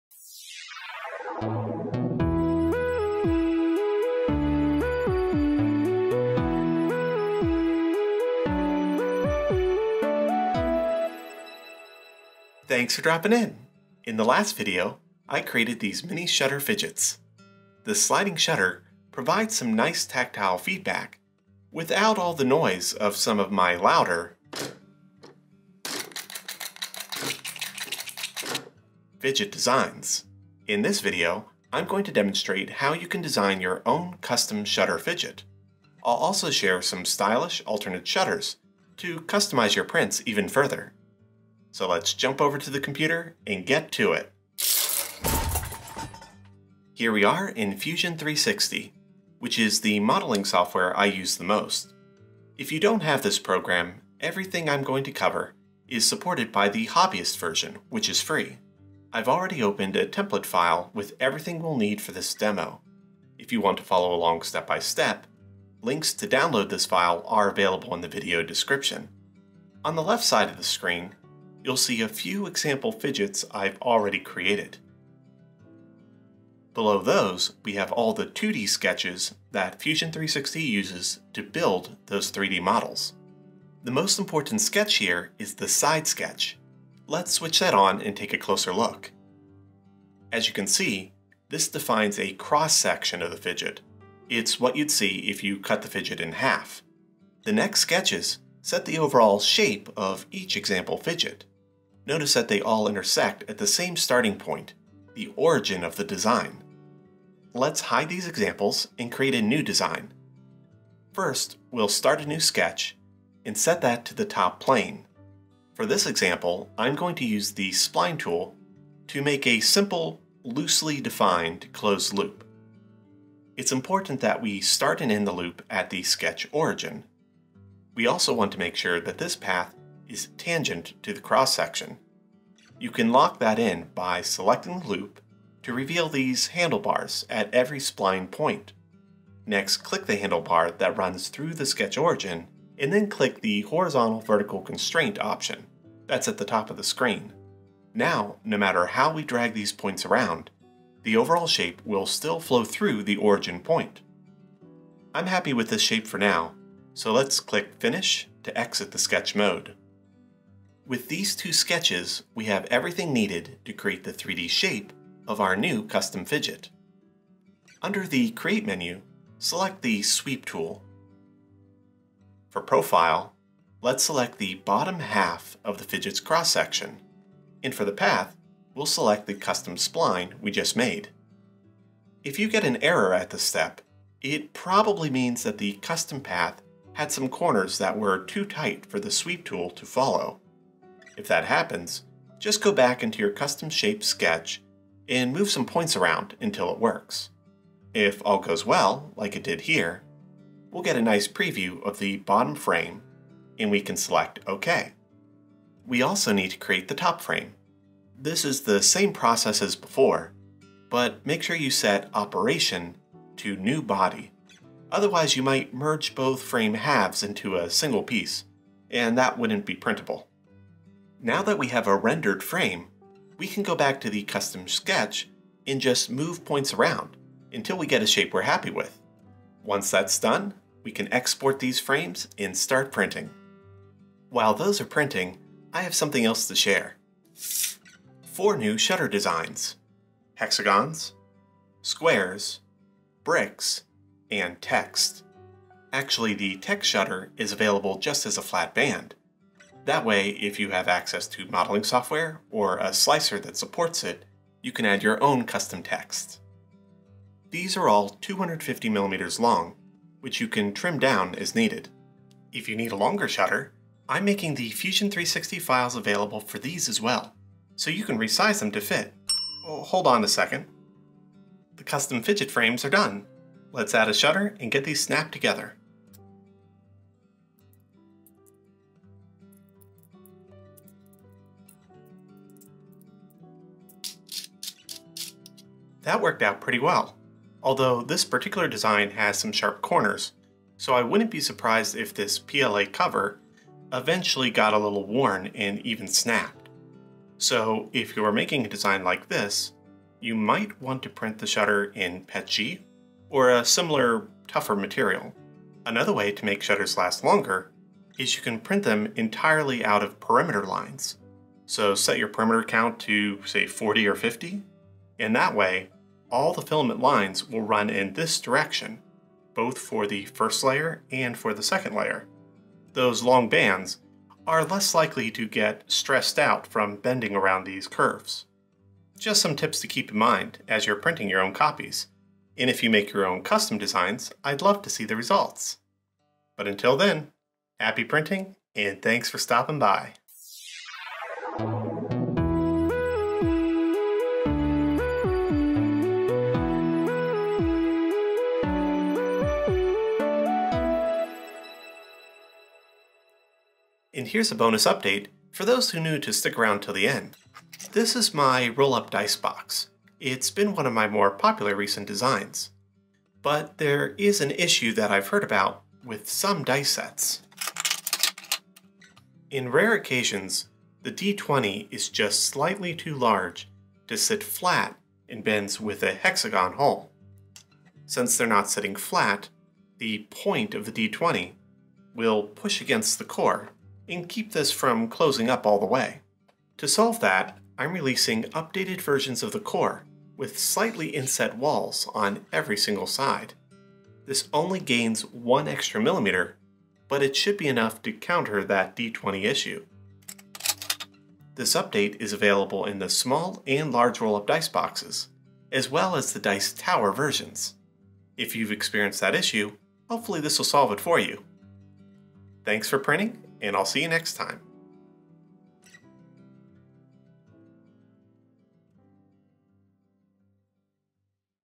Fusion 360 - Design A Custom Shutter Fidget
In this video, we design and print a custom shutter-based fidget toy.

All the required files to make your own are available to download.

Happy printing!

== Featured Printy ==

== Materials ==
Protopasta PLA
Inland PLA & PETG
Prusament PLA
Cookiecad PLA

== Credits ==

Slogra